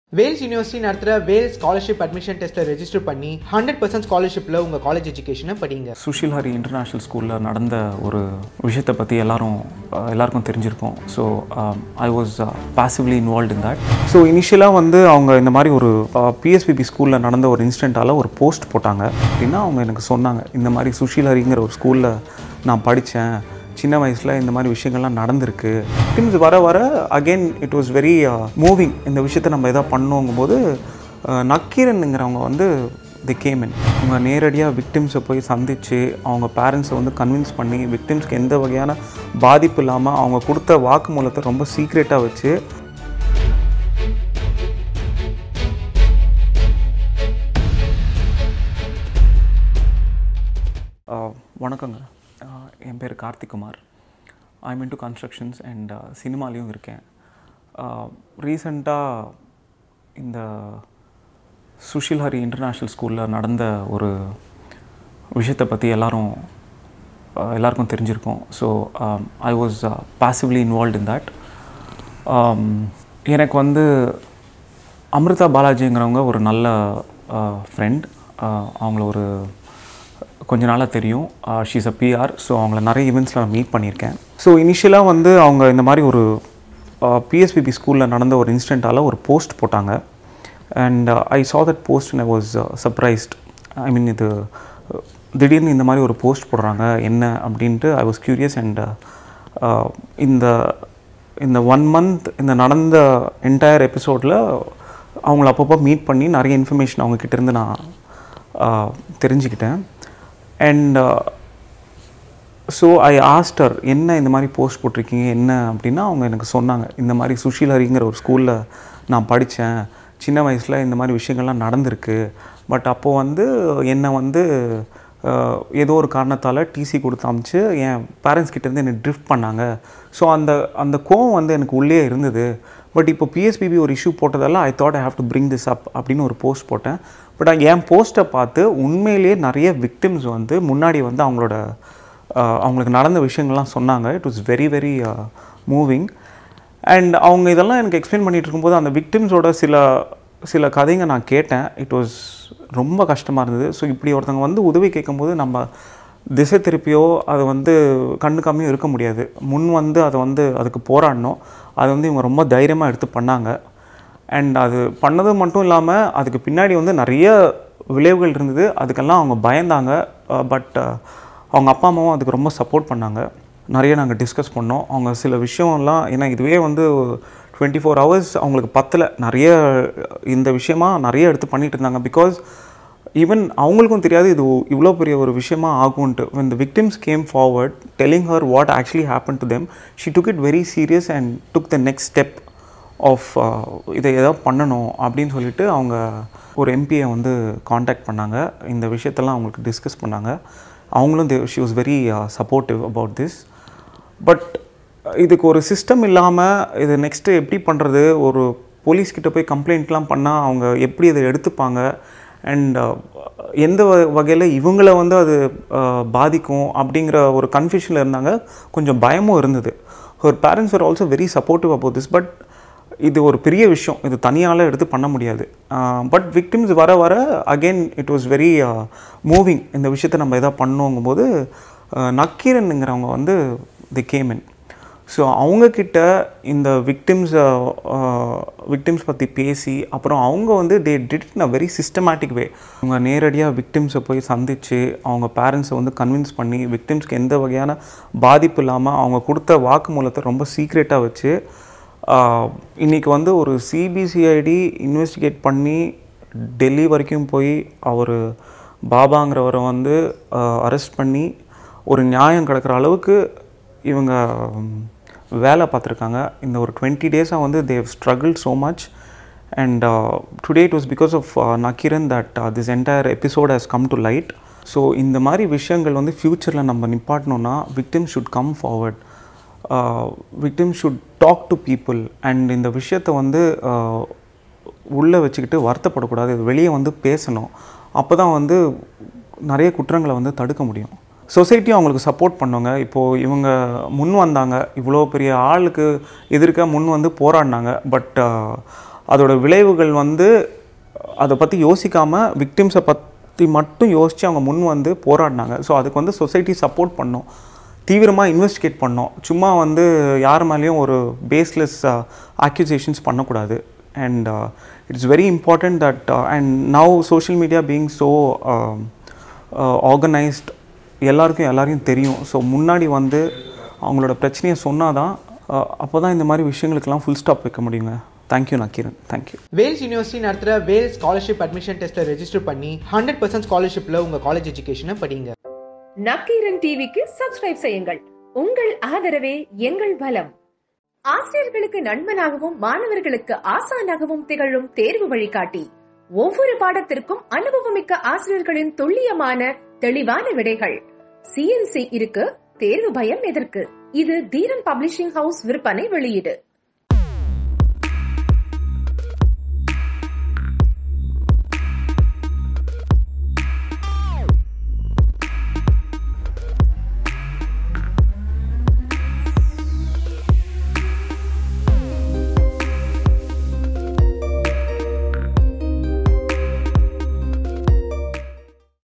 VICTIMS கதைய கேட்டாலே...! KARTHIK ABOUT SIVA SANKAR BABA SUSHILHARI INTERNATIONAL SCHOOL
VELS SCHOLARSHIP ADMISSION TEST and get upto 100% Scholarship in Tuition fee offered by VISTAS.